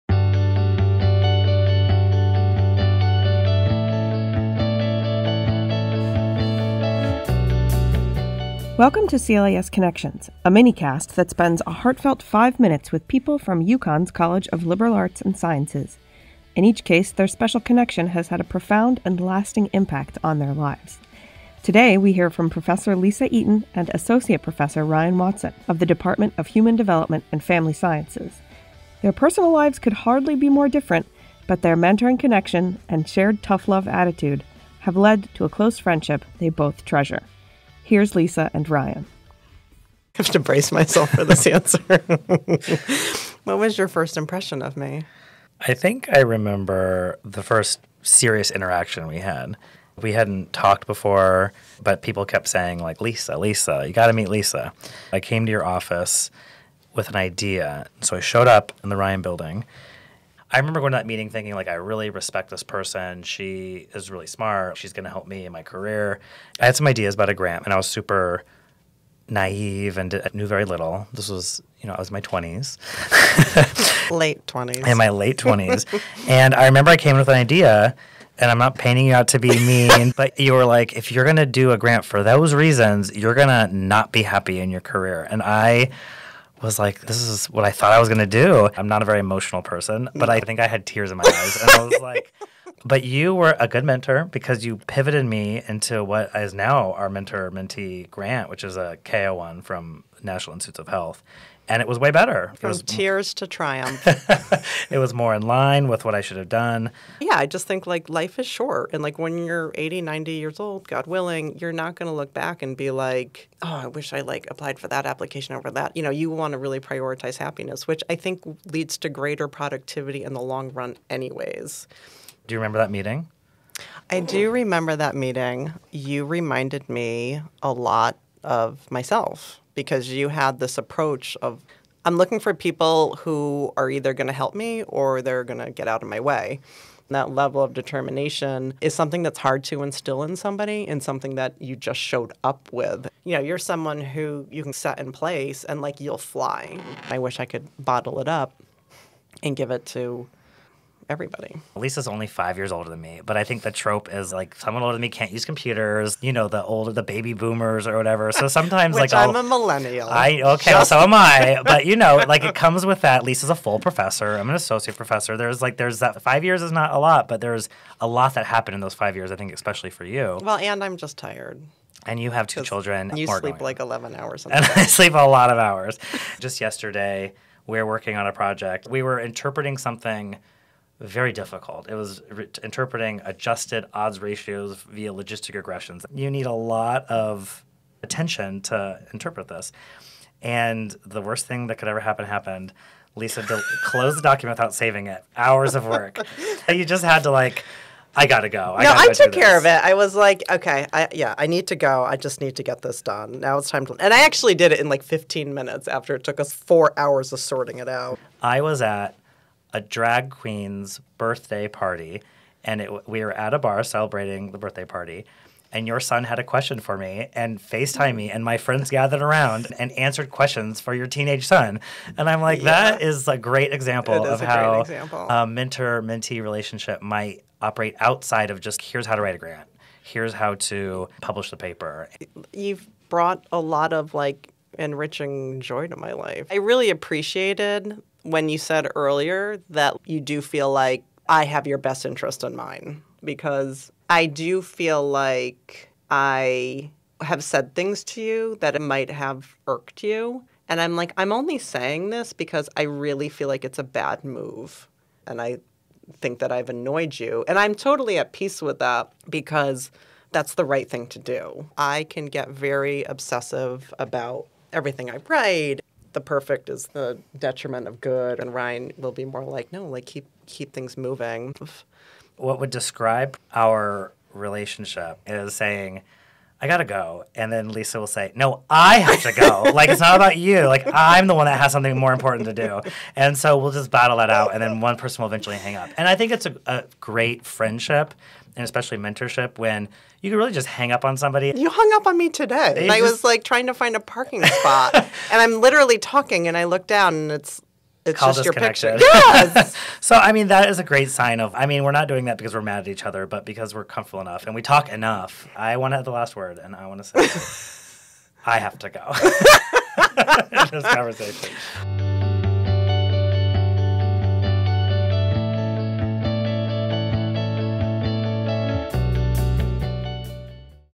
CLAS Connections: Lisa Eaton and Ryan Watson
Professor Lisa Eaton and Associate Professor Ryan Watson discuss the connection they built in the Department of Human Development and Family Sciences — and the importance of mentorship in the world of academia.